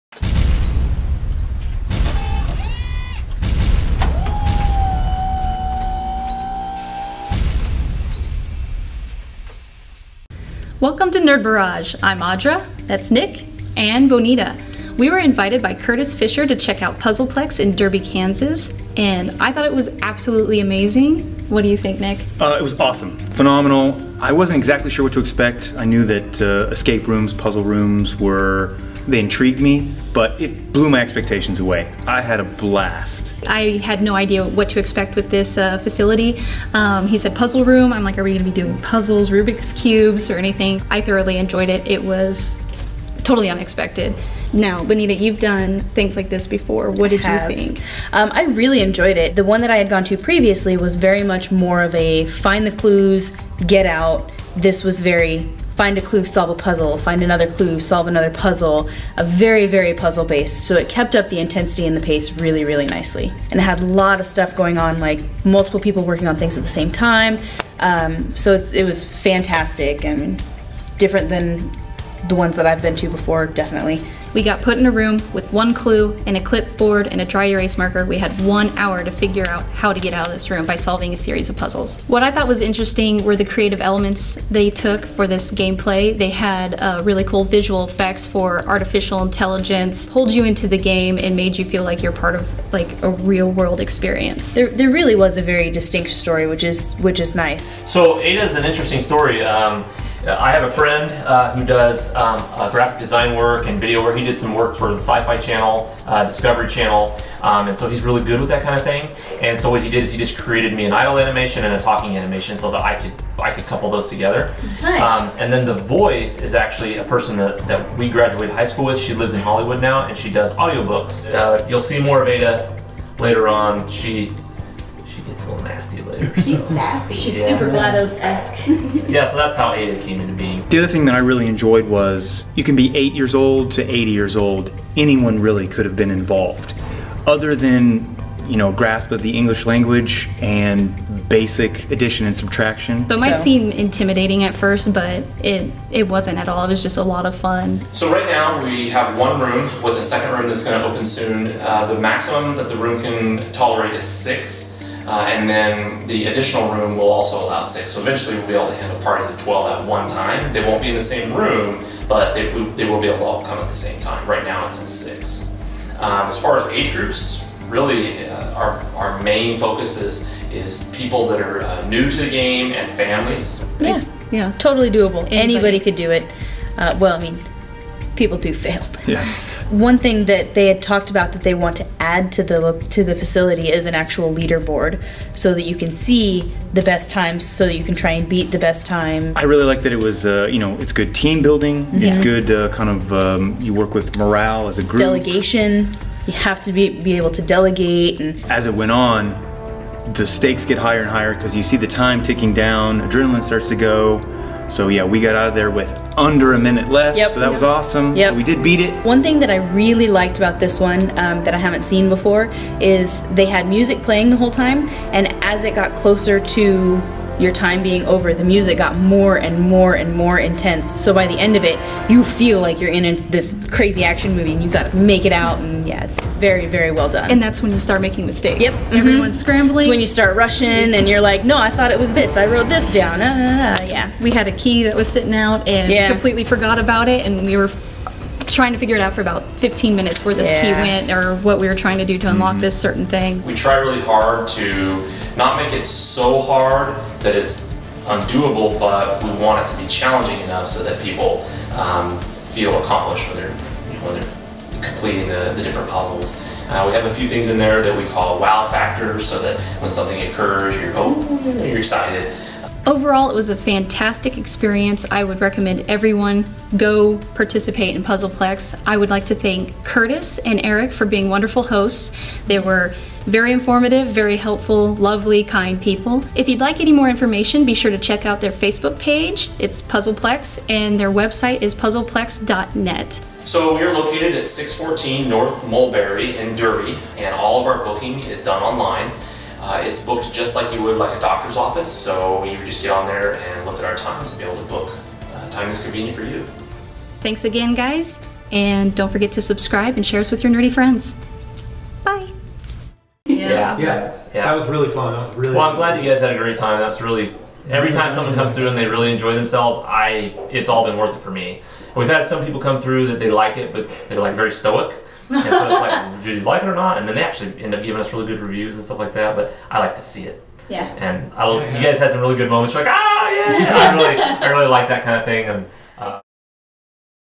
Puzzle Plex Review
Nerd Barrage visits Curtis and Eric at the Puzzle Plex escaped room in Derby Kansas for some fun and a review!